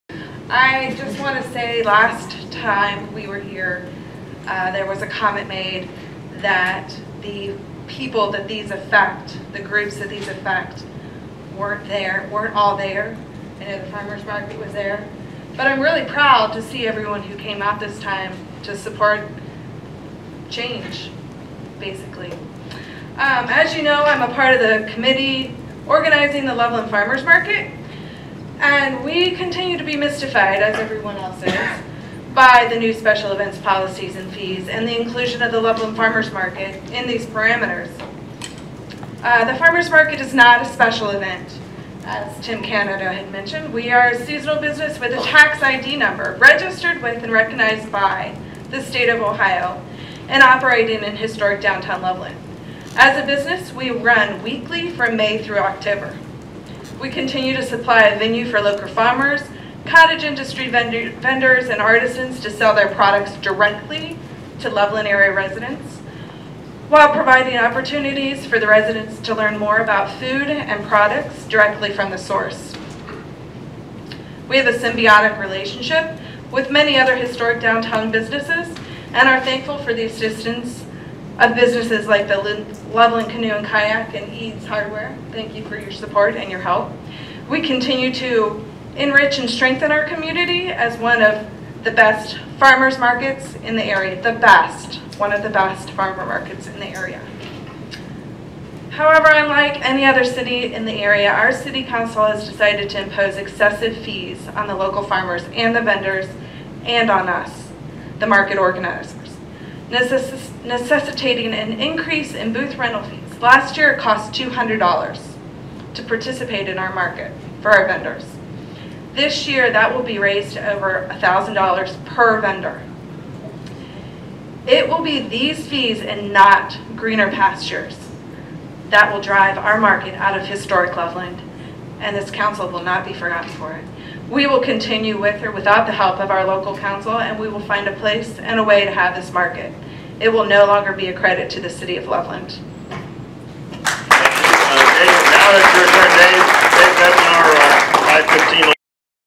Emily Hicks asking City Hall to re-consider public event policy and fees
The public showed up and overflowed into the hallways at the January 24 Loveland council meeting to show displeasure with new fees and terms for holding "Public Events" on private or public property in the city of Loveland.